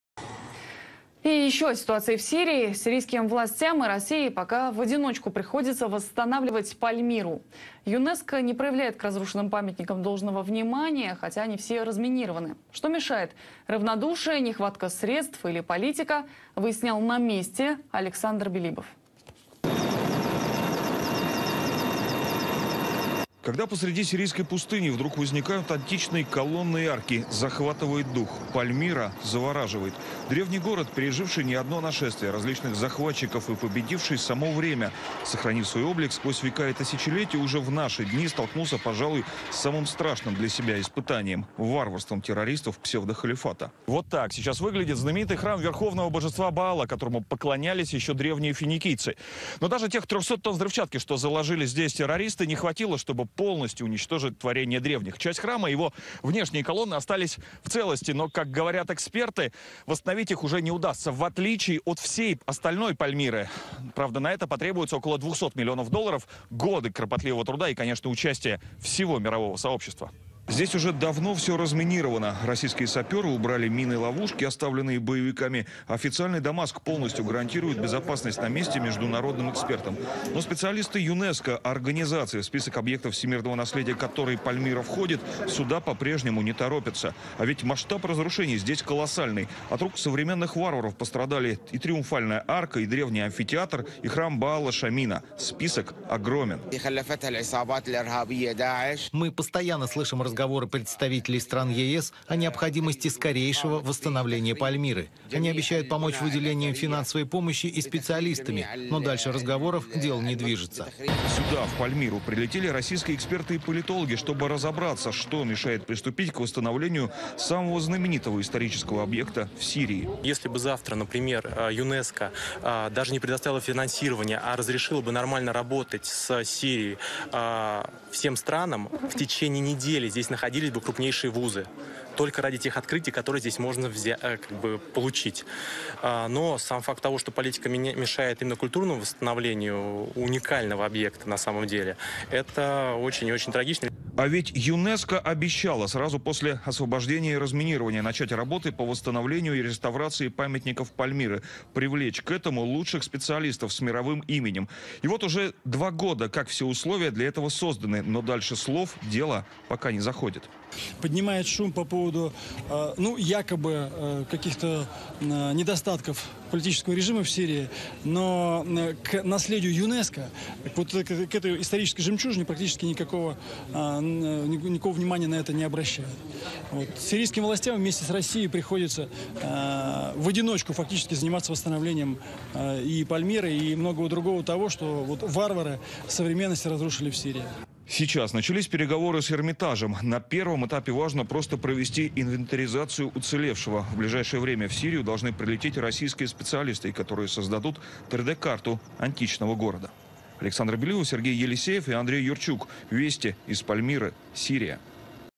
Economic blockade obstructs the restoration of Syria's Palmyra
There has been no real international or UNESCO support for the restoration of Palmyra in the past two years, due to political and economic 'sanctions' obstruction. Syrian and Russian authorities will have to do it alone.